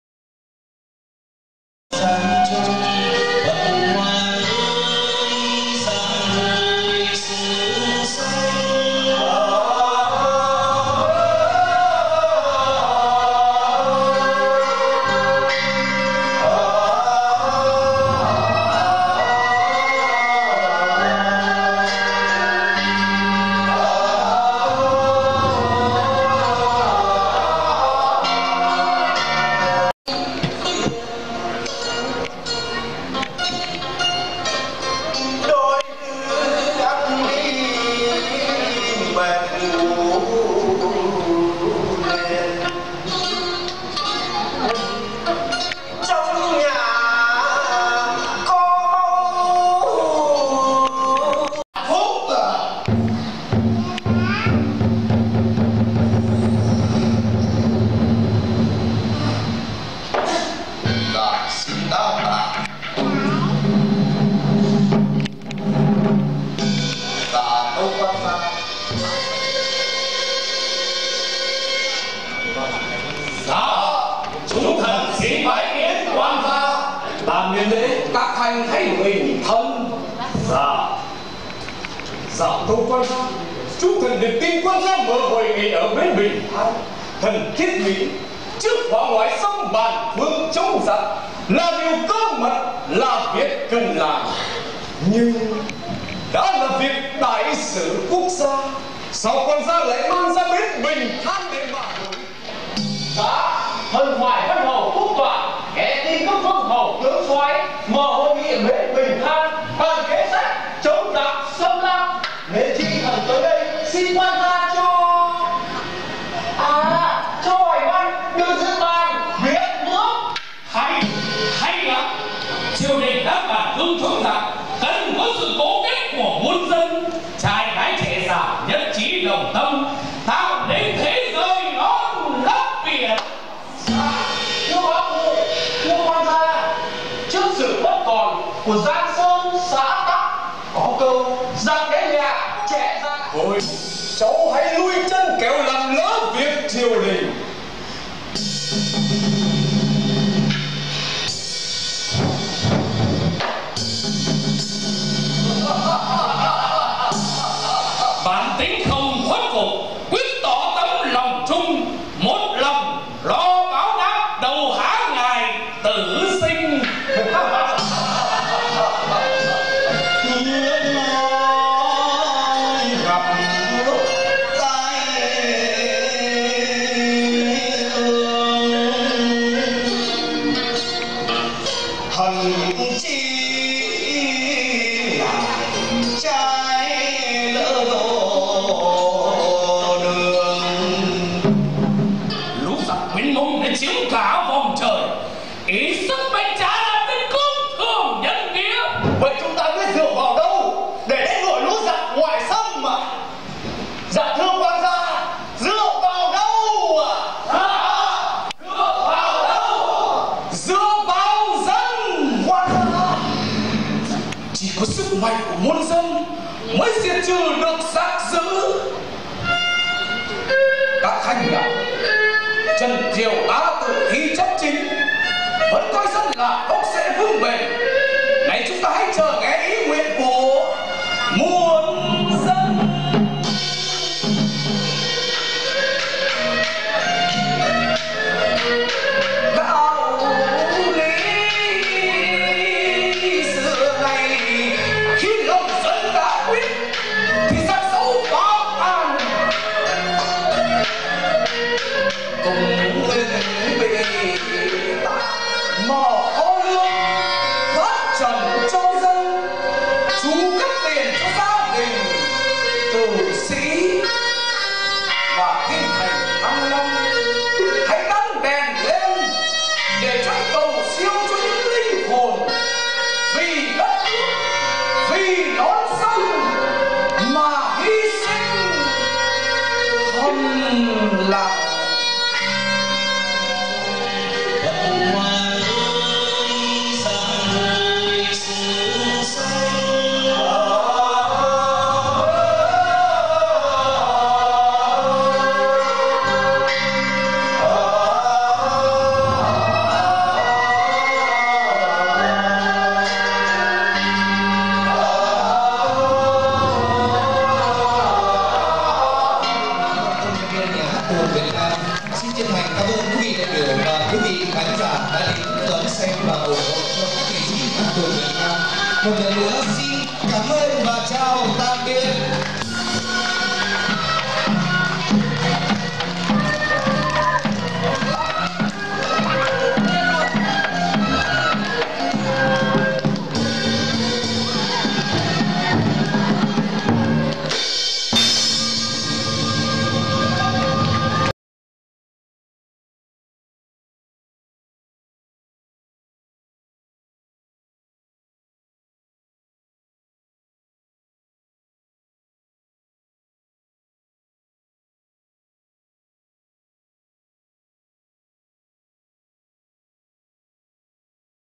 PHẬT HOÀNG TRẦN NHÂN TÔNG (Trích ) DUC HOA
Hôm nay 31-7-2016 Đi xem tuồng Phật Hoàng Trần Nhâ Tông ở nhà hát Hòng Hà Hà nội DUC HOA